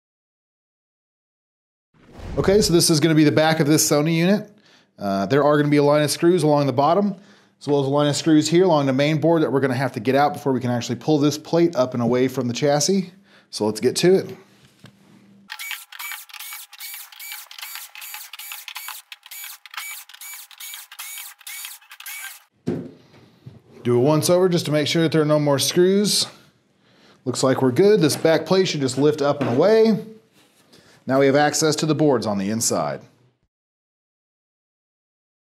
How to remove the back of your TV. (SONY XBR-65X850E)
Removing the back to your TV is the first step in fixing your unit.  It is important to know how to properly take it off, so you can prevent breaking the case.

Hours: Mon-Fri, 8:00AM-4:30PM CST